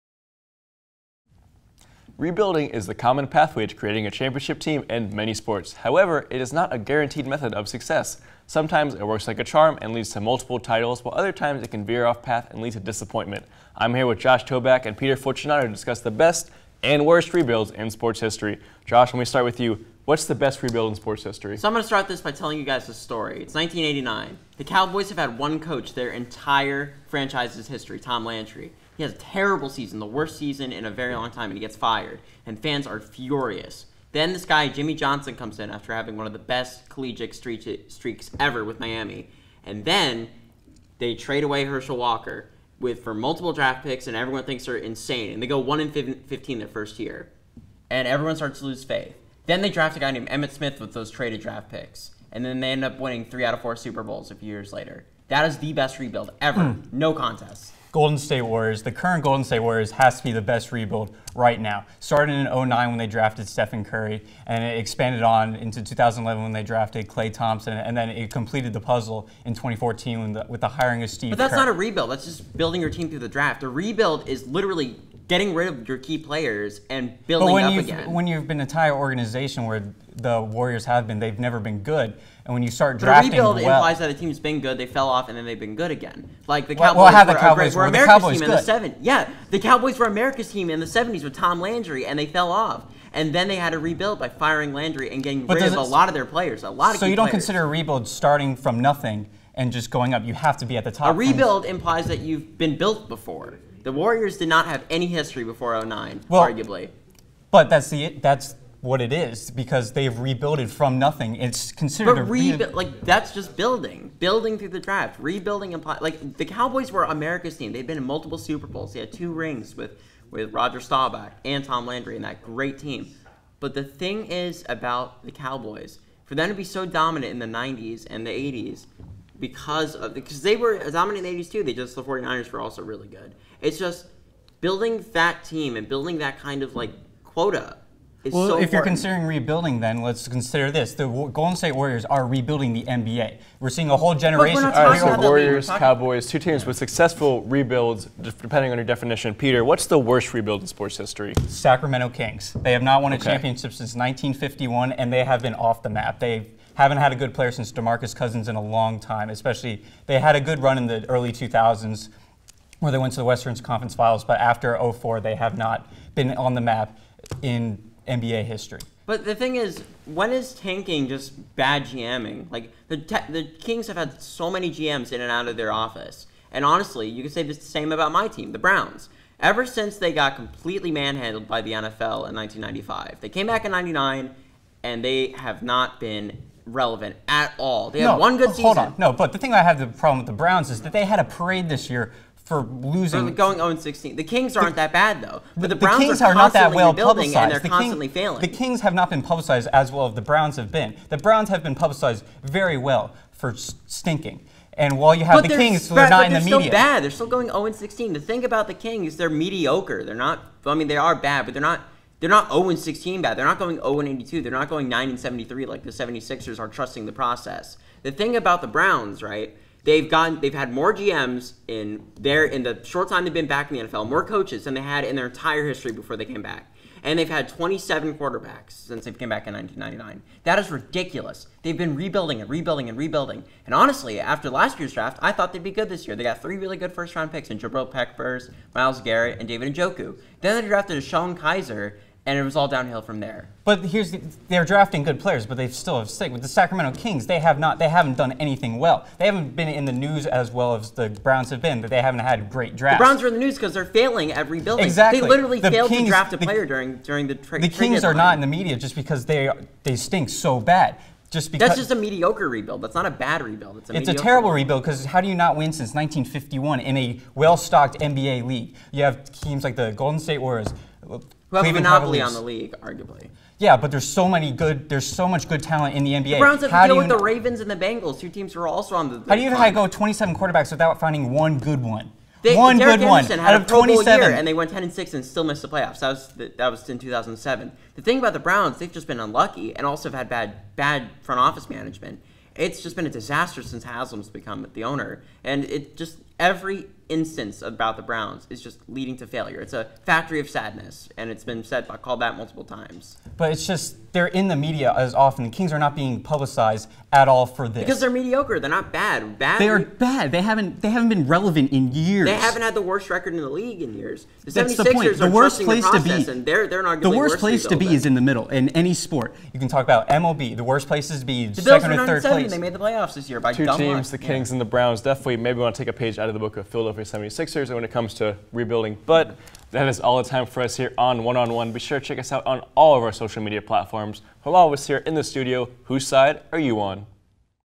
Week 2: Webcast Best and Worst Rebuild
Teams choose to rebuild every season. Usually opting to focus on young talent development to win a championship in the far future. But sometimes all doesn't go to plan. Josh Toback and Peter Fortunato join Matt to break down the best and worst rebuilds in sport history.